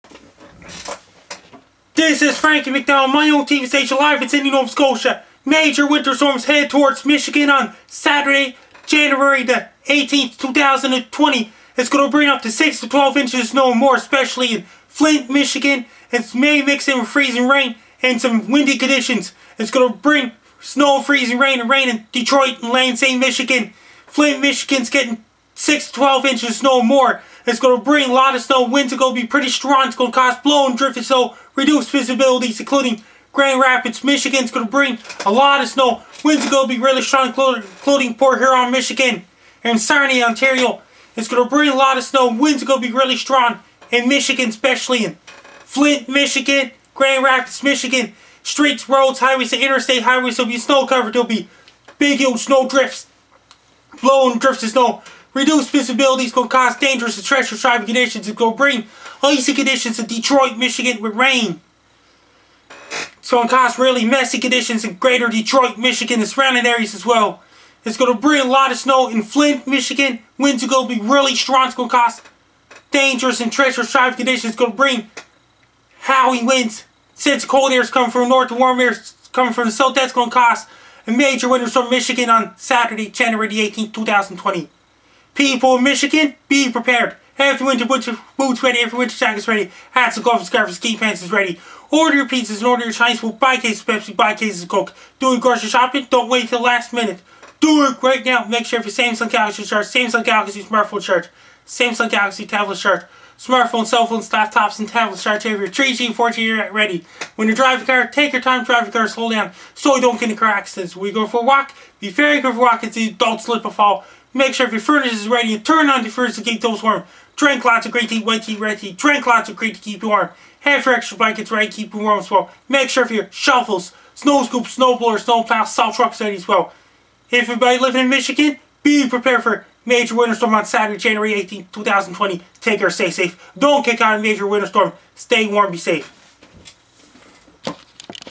Major Winter Storm to Hit Michigan on Saturday January 18, 2020
Major Winter Storm is on its way for Michigan on Saturday January 18, 2020 and it will bring 6 to 12 Inches of Snow or More and it will bring Lots of Snow Especially in Flint Michigan and in Detroit it will bring Snow Mixed Freezing Rain and then Rain Same with Lansing Michigan it will bring lots of Snow in Grand Rapids Michigan and it will bring Blowing and Drifting Snow and it will Cause Reduced Visibility and it will bring Dangerous and Treacherous Driving Conditions and Streets, Roads, Highways and Interstate Highways will be Snow Covered and it will bring a lot of Snow and Rain in Detroit and Windsor Ontario and Port Huron Michigan including Sarnia Ontario and it will be a lot of Snow in Flint Michigan and Grand Rapids Michigan and it will bring Howling Winds and cold air is coming from the north and warm air is coming from south that will Cause a Major Winter Storm in Michigan.  People in Michigan Be Prepared Have your Winter Boots, Winter Jackets, Hats, Gloves, Scarfs and Ski Pants Ready.  Do your Grocery Shopping Don't Wait until the Last Minute Do it Right Now.  Have your Samsung Galaxys, Samsung Galaxy Smartphones, Samsung Galaxy Tablets, Smartphones, Cell Phones, Laptops and Tablets Charged and Have your 3G and 4G Internet Ready.  When you are Driving your Car Take your Time Driving your Car and Slow Down so you Don't Get in the Car Accident.  When you are Walking Be Very Careful while you are Walking so you Don't Slip and Fall.  Have your Furnaces Ready and Turn on the Furnaces to Keep the House Warm.  Drink Lots of Green Tea, White Tea, Red Tea and Drink Lots of Green Tea to Keep you Warm.  Have your Extra Blankets Ready to Keep you Warm.  Have your Shovels, Snow Scoops, Snow Blowers, Snow Plows and Salt Trucks Ready.  If you Have anybody Living in Michigan Be Prepared for Major Winter Storm on Saturday January 18, 2020.  Take Care and Stay Safe and Don't Get Caught in the Major Winter Storm Stay Warm and Be Safe.